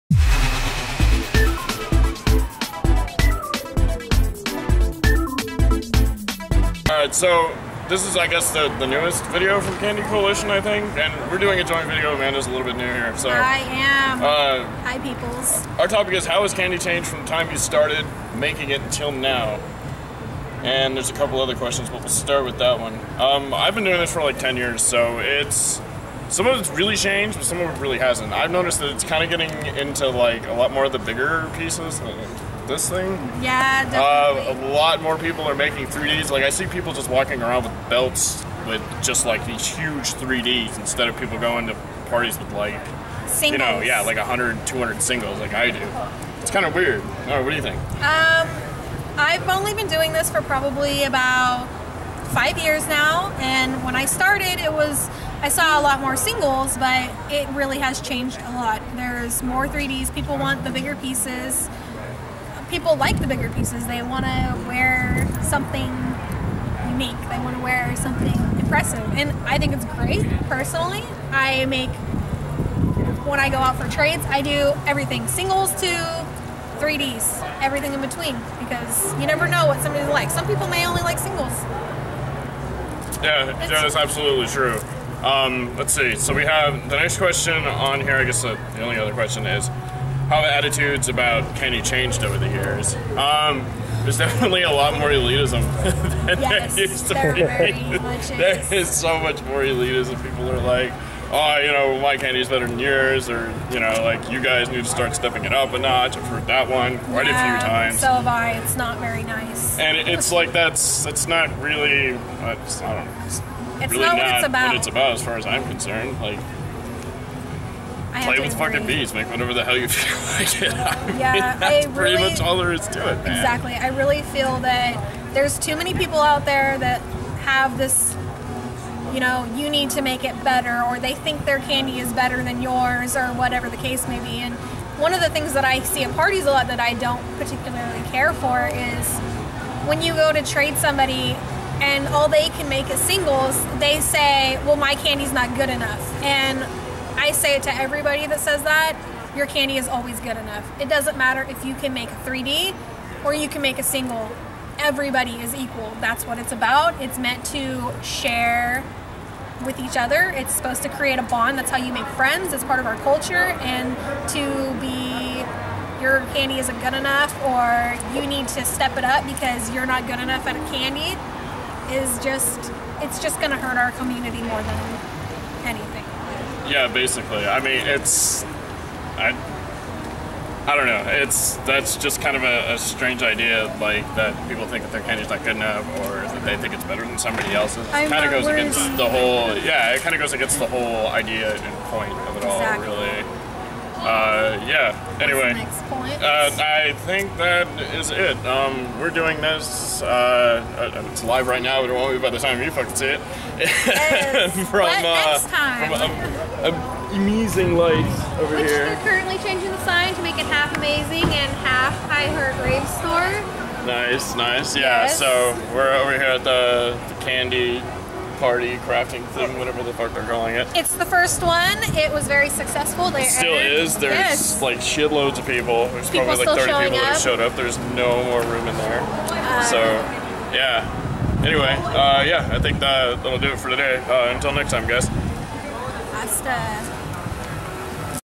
Orgasmo & MzAmandaz - What's Changed About Kandi?
Special Edition Joint Video with Orgasmo and MzAmandaz!

Recorded at emazinglights.com 's own retail store in Los Angeles during their first ever Kandi Krafting Session! Super fun stuff!

Today's Topic : How has kandi changed from the time you started making it until now? How have the attitudes about kandi changed?

Shoutout to my buddy Hinz ) for being our camera man on this! Love you man!